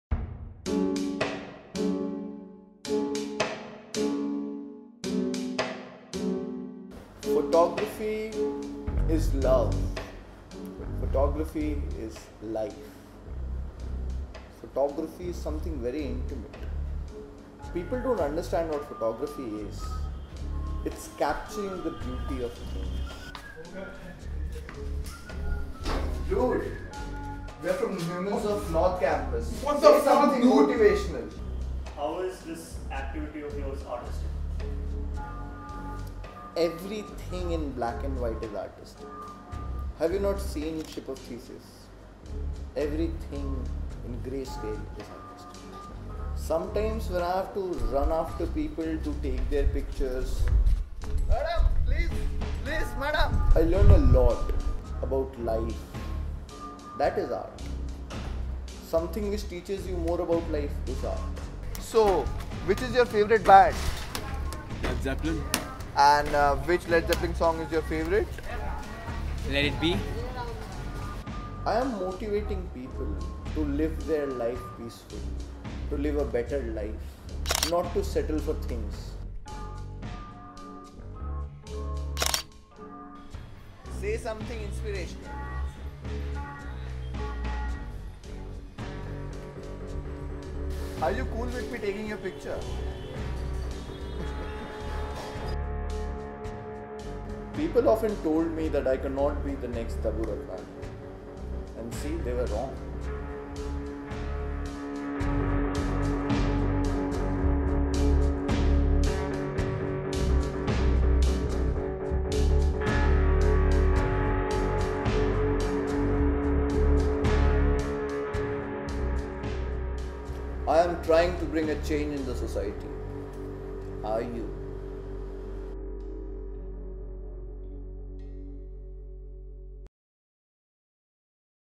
Humans Of Delhi University.
A parody of the famous Humans of New York, the film follows Bhatindon Sheikh, a wannabe photographer.